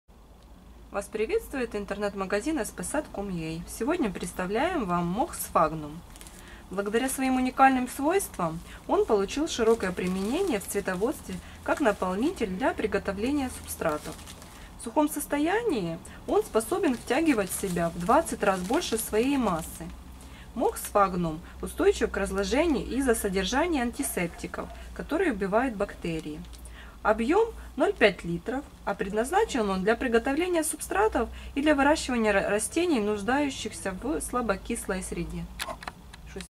МОХ сфагнум
Благодаря своим уникальным свойствам сфагнум получил широкое применяется в цветоводстве как наполнитель для приготовления субстратов.